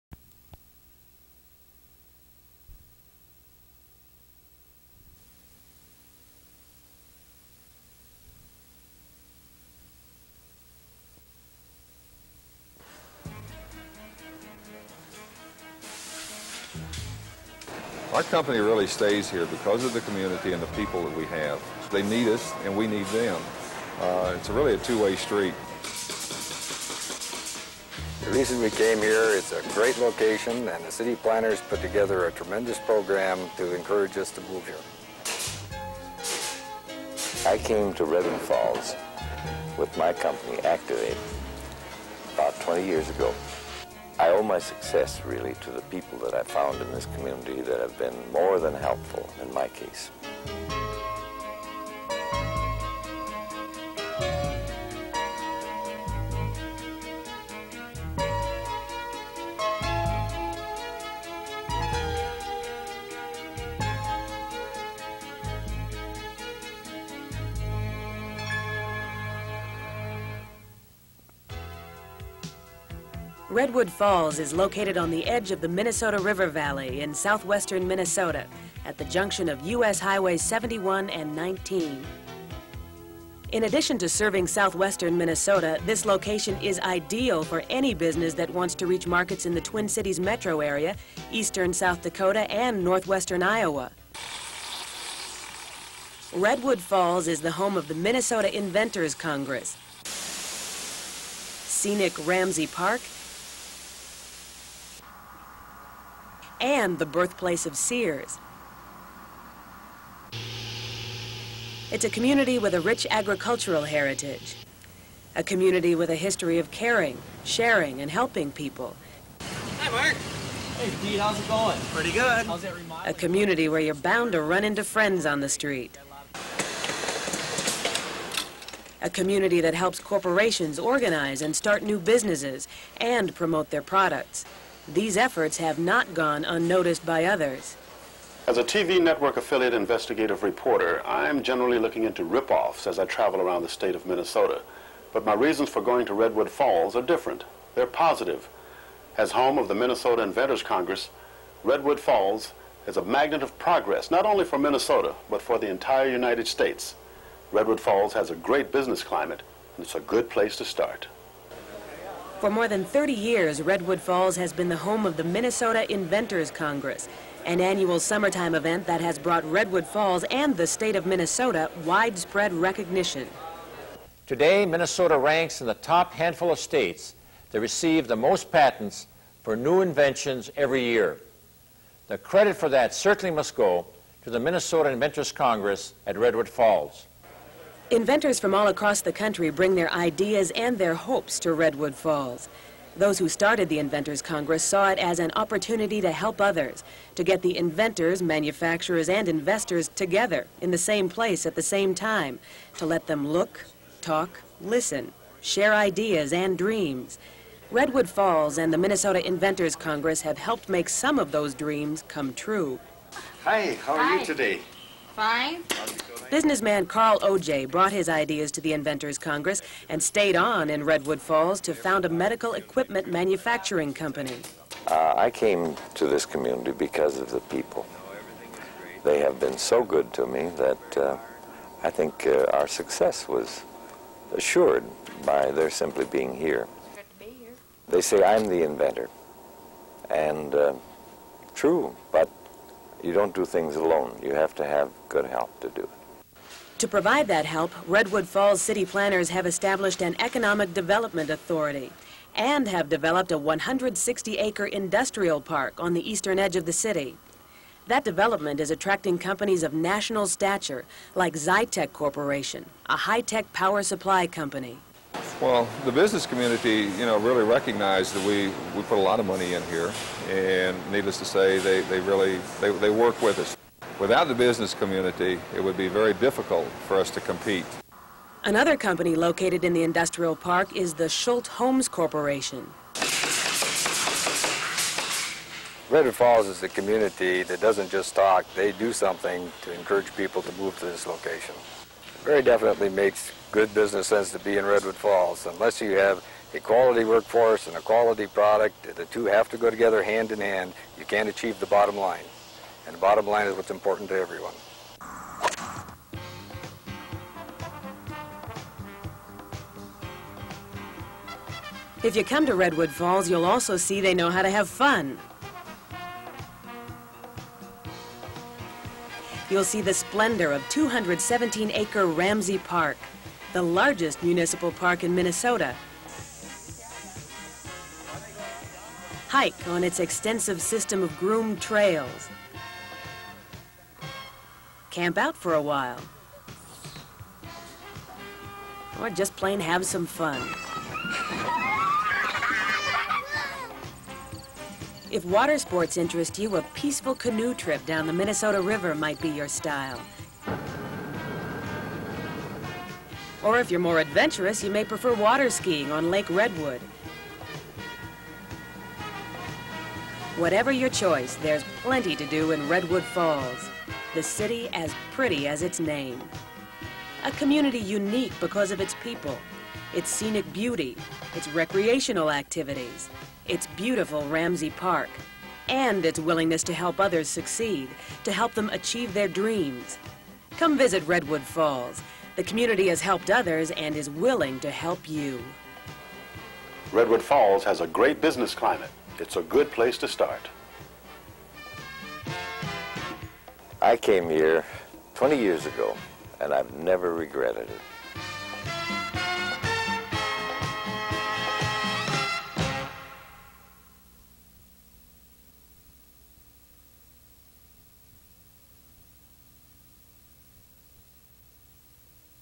Redwood Falls, a Good Place To Start.flv
View a historic perspective of the Minnesota Inventors Congress invention expo and the community that has hosted it for 54 years. Some items may be dated, but the heart of the story is still true today!